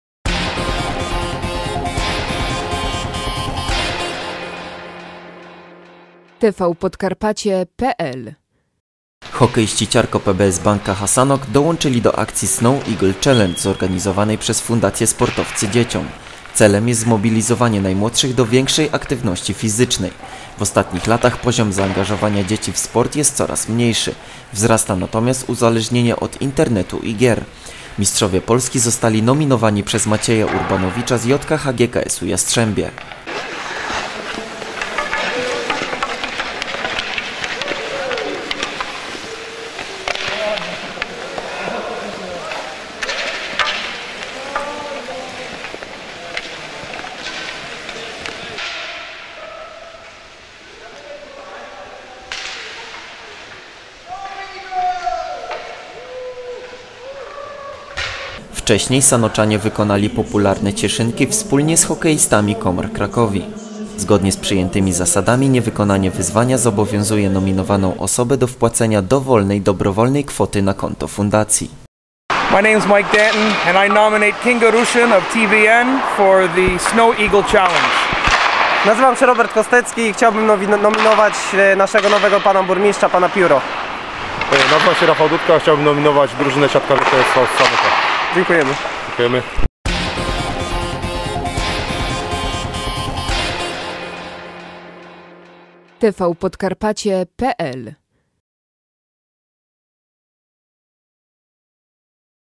Sanoccy hokeiści w akcji Snow Eagle Challenge. Zobacz kogo nominowali!
SANOK / PODKARPACIE. Hokeiści Ciarko PBS Bank KH Sanok dołączyli do akcji Snow Eagle Challenge zorganizowanej przez fundację Sportowcy Dzieciom. Wszyscy zawodnicy mistrza Polski położyli się na lodzie i wykonali popularnego orła. Na koniec Mike Danton, Robert Kostecki i Rafał Dutka nominowali kolejne osoby do wzięcia udziału w zabawie.